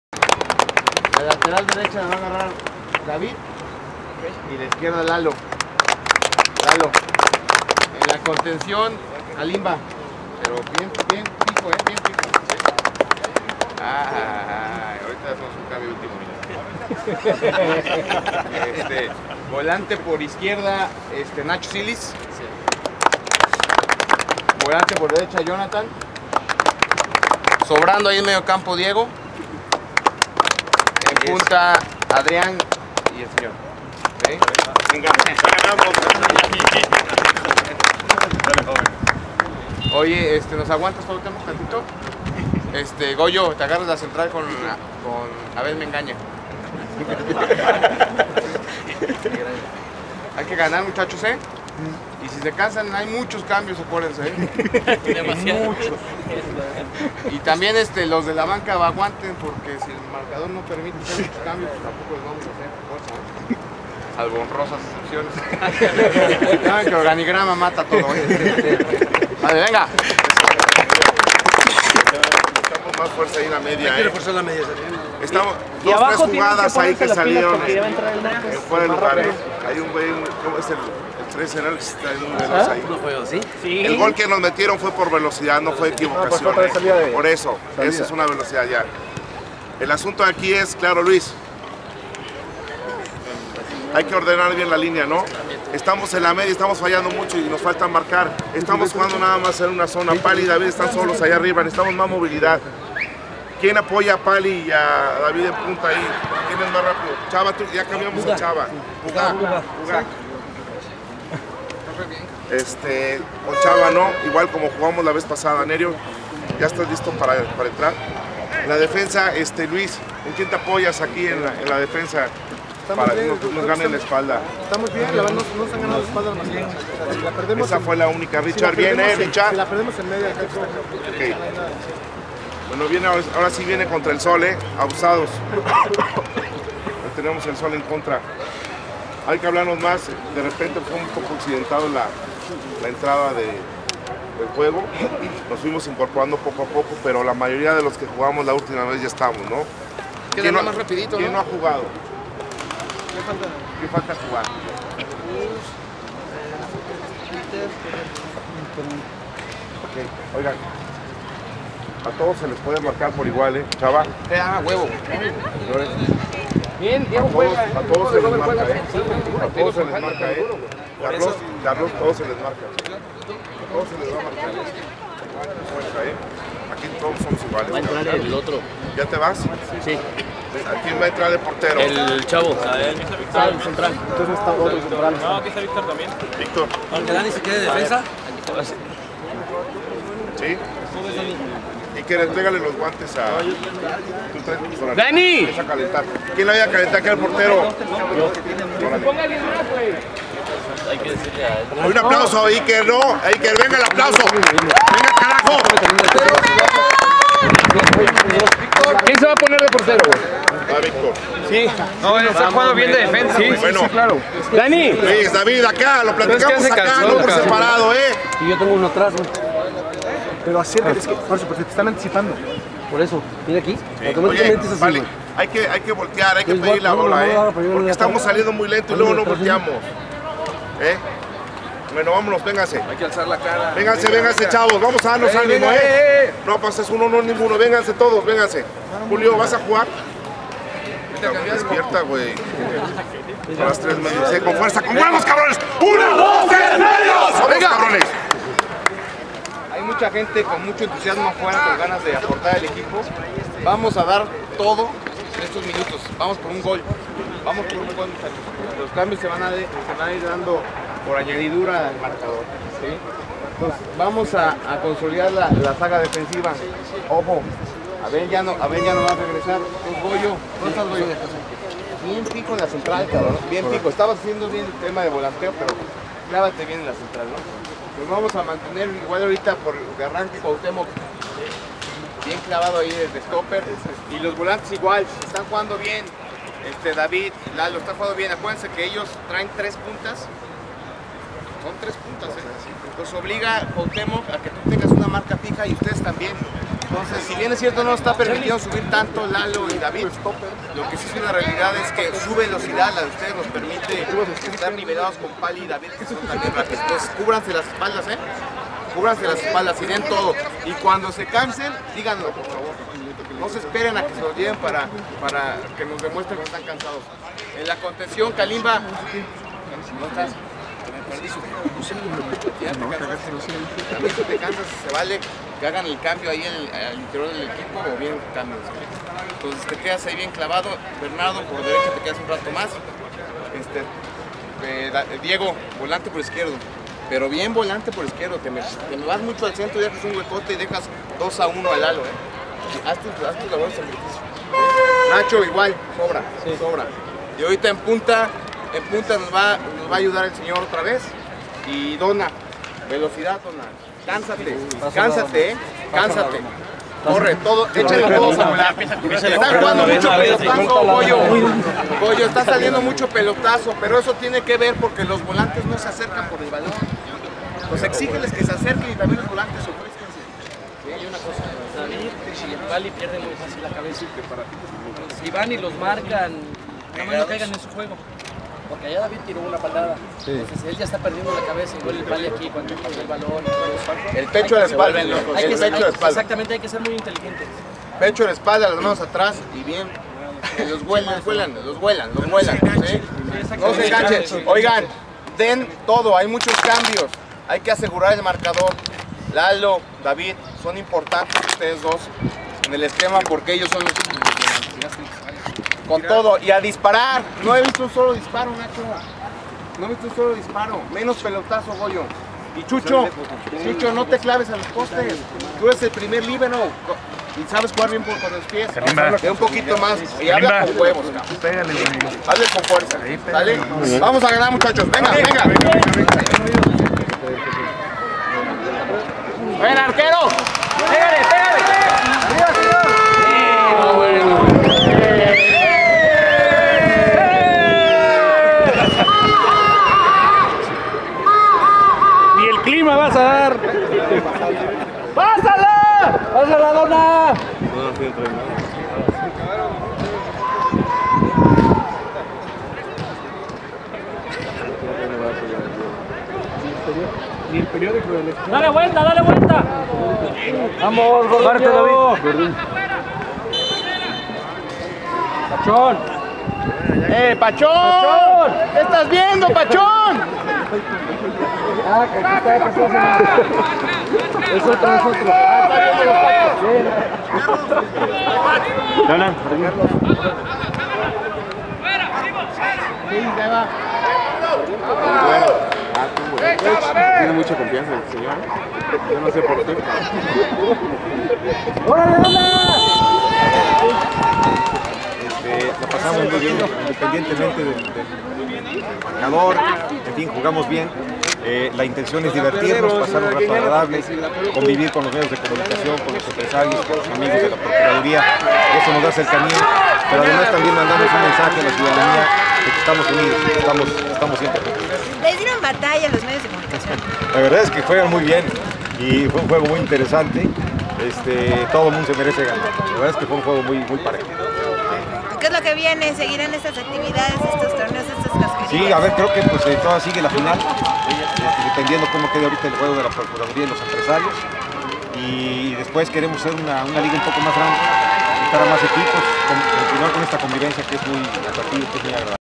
PEPE CALZADA-EL GOBERNADOR DE QUERÉTARO JUGANDO FUTBOL VS MEDIOS 27OCT2012.
EL GOBERNADOR DEL ESTADO DE QUERÉTARO JOSE CALZADA JUGANDO FUTBOL EN CONTRA DE LOS MEDIOS A LOS CUALES DERROTARON 2 A 0 Y ASI PASARON A LA FINAL EL PROXIMO DOMINGO.